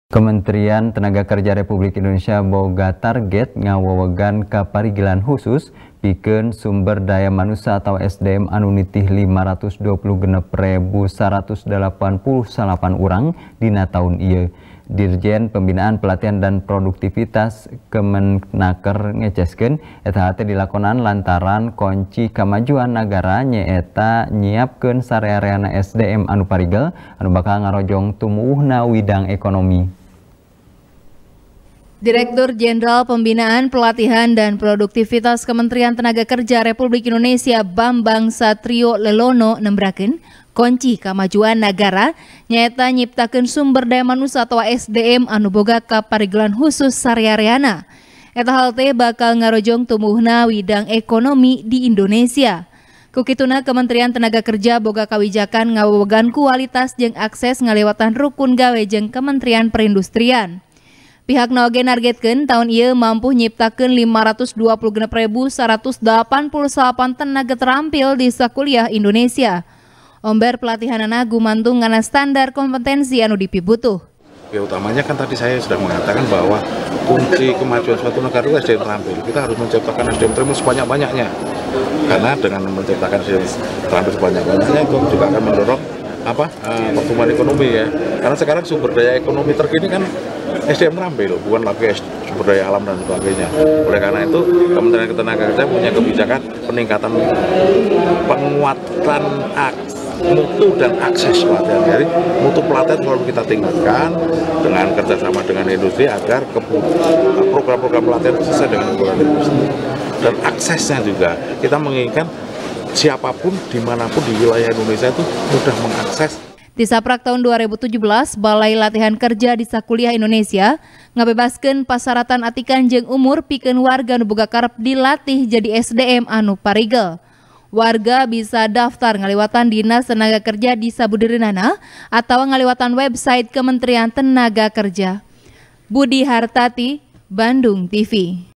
Kementerian tenaga kerja boga target 526.189 urang boga kaparigelan husus | TP | BANDUNGTV
TP | BANDUNGTV | IWAN M RIDWAN | Direktur Jendral Pembinaan, Pelatihan, dan Produktivitas Kementerian Tenaga Kerja Republik Indonesia, Bambang Satrio Lelono nembrakeun, konci kamajuan nagara nya eta nyiptakeun sumber daya manusa atawa sdm anu boga kaparigelan husus sarea-reana. Eta hal teh bakal ngarojong tumuwuhna ekonomi Indonesia. Ku kituna, kementerian tenaga kerja boga kawijakan ngawewegan kualitas jeung akses ngaliwatan rukun gawe jeung kementerian perindustrian. Pihakna oge nargetkeun, taun ieu mampuh nyiptakeun 526 rebu 189 tenaga terampil di sakuliah Indonesia. Omber pelatihanana gumantung kana standar kompetensi anu dipibutuh.